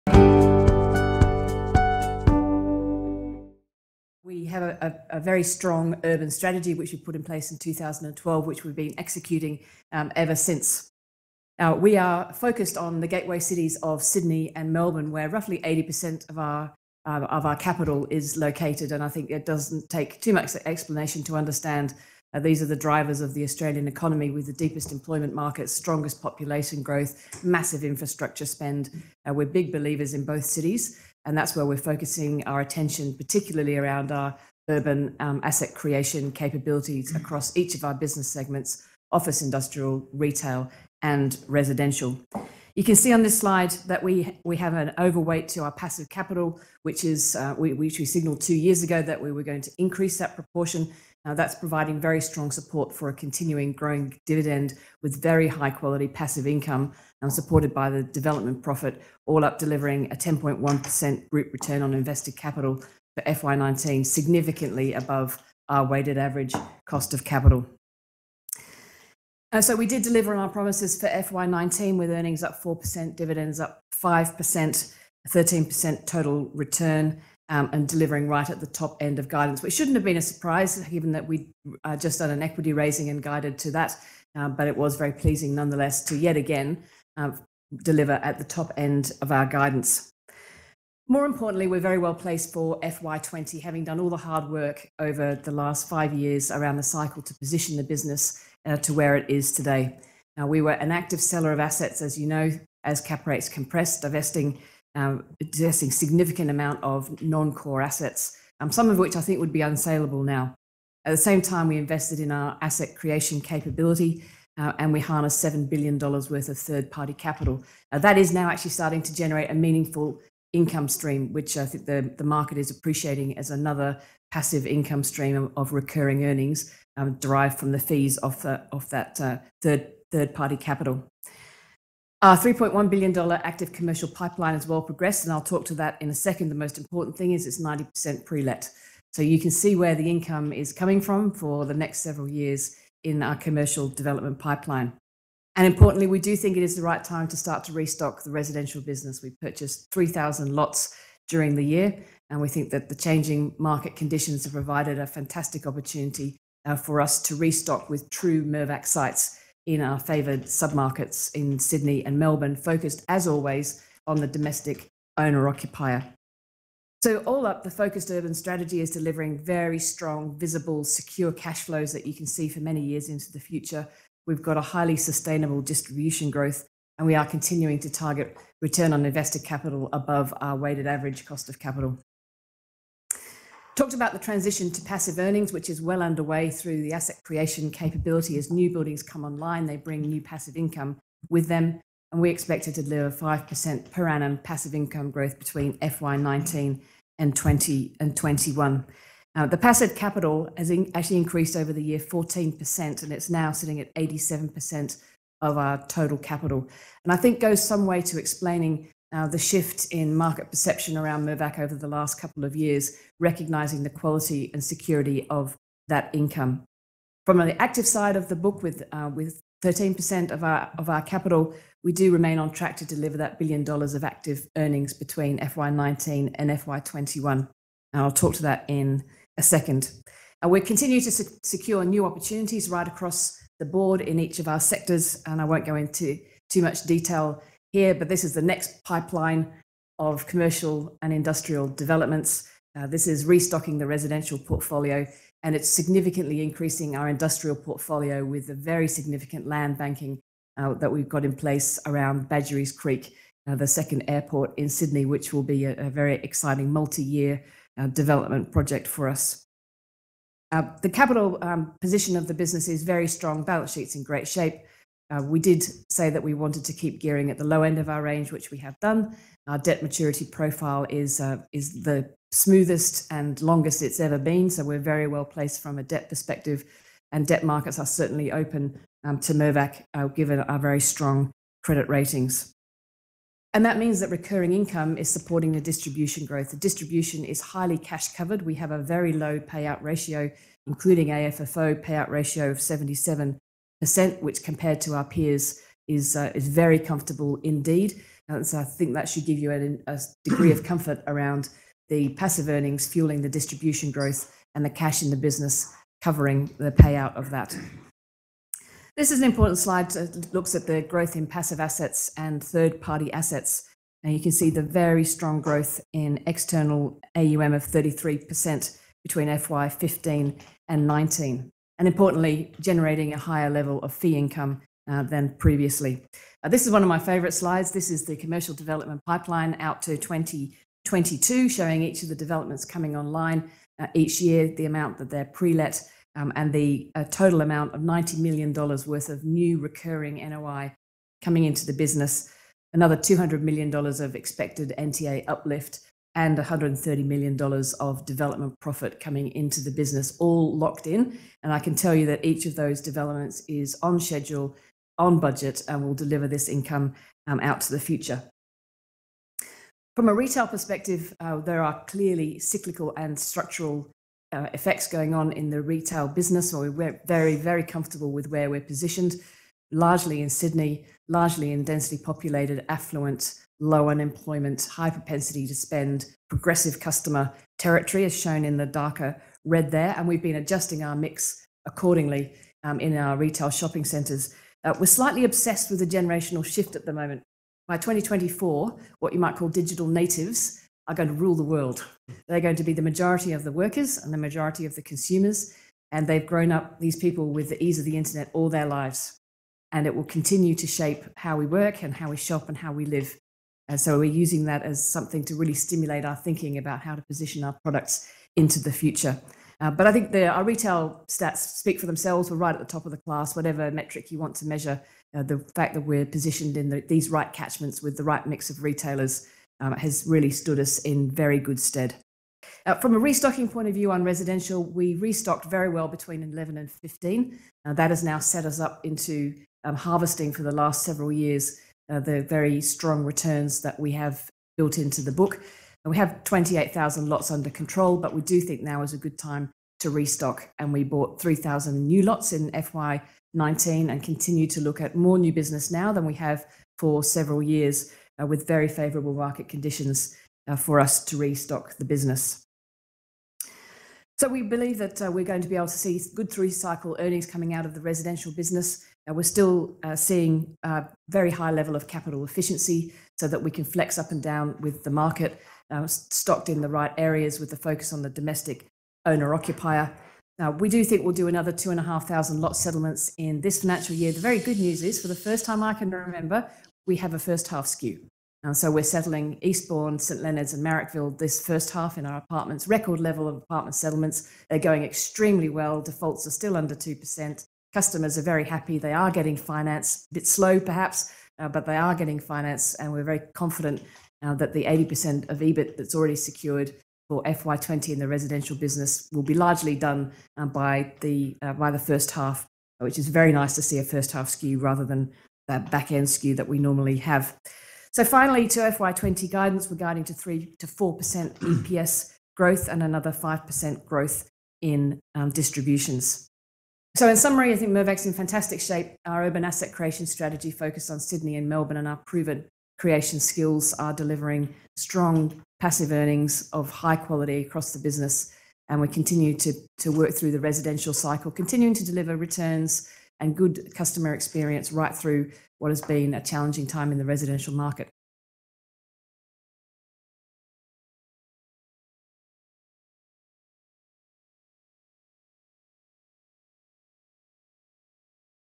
Mirvac Group (ASX:MGR): Susan Lloyd-Hurwitz, Managing Director & Chief Executive Officer
Susan Lloyd-Hurwitz, Managing Director and Chief Executive Office of Mirvac Group (ASX:MGR) discussed the company's FY19 results with the Morgans Network